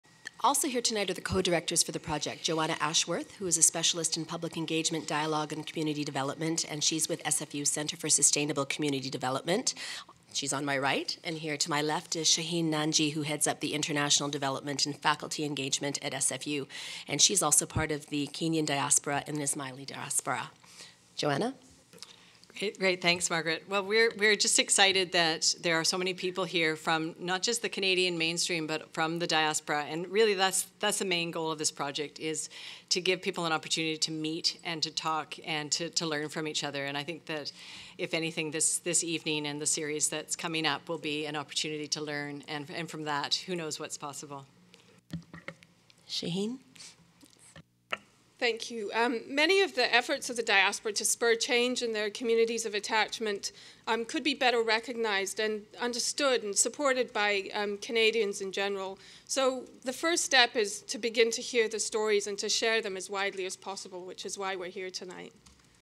1-3: Joanna Ashworth and Shaheen Nanji - Welcome
Welcome:
Setting the Stage for Dialogue on Diaspora in Development

Joanna Ashworth and Shaheen Nanji are the Co-Directors of the Engaging Diaspora in Development project.

Part of the "Innovations in Poverty Reduction and Economic Development" dialogue from the Engaging Diaspora in Development series.